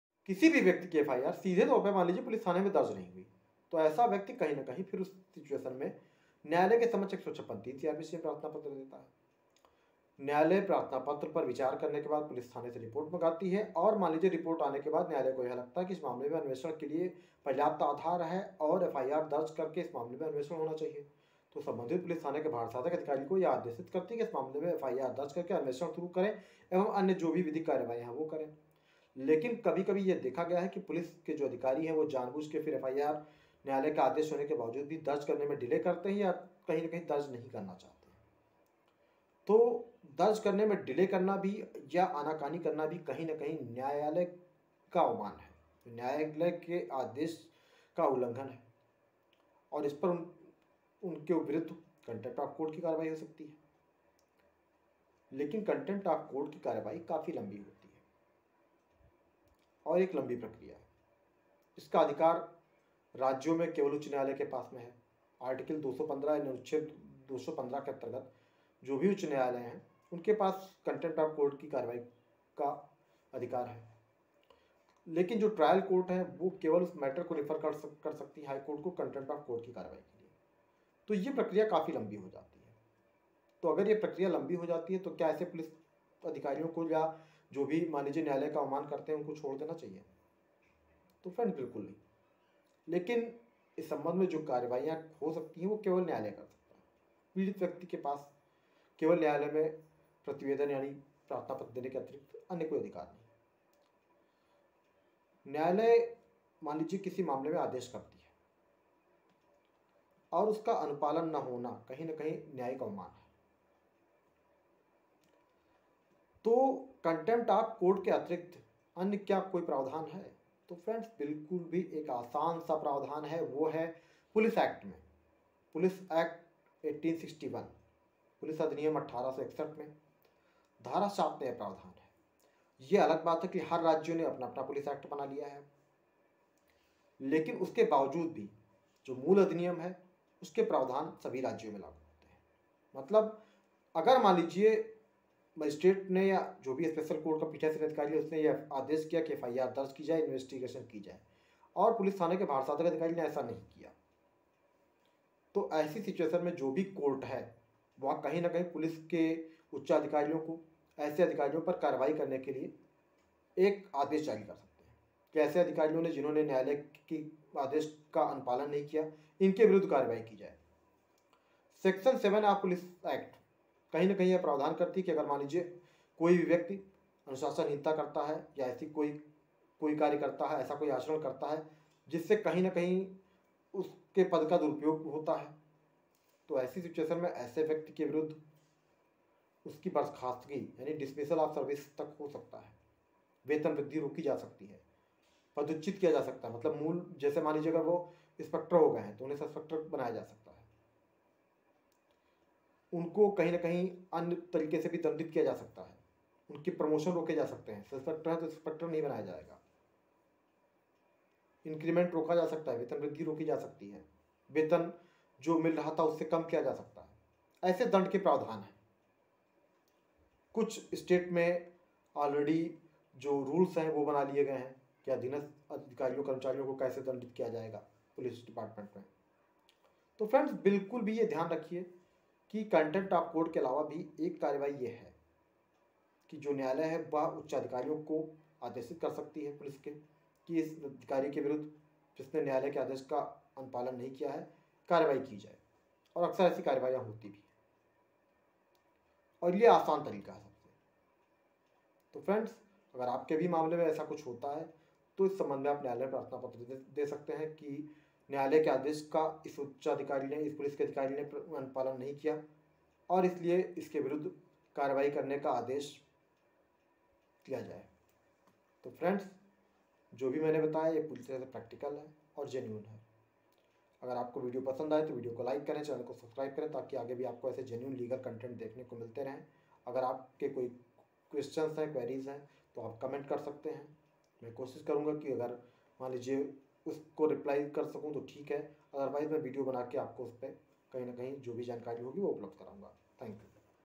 #156(3)CrPC से FIR दर्ज करने का,पुलिस तब भी न दर्ज करे FIR तब क्या करे?
156(3)CrPC के प्रार्थना पत्र पर न्यायालय ने पुलिस को दिया FIR  दर्ज करने का आदेश, पुलिस न्यायालय के आदेश के बाद भी FIR to दर्ज न करे तो क्या करे, जानने के लिए पूरा वीडियो देखिए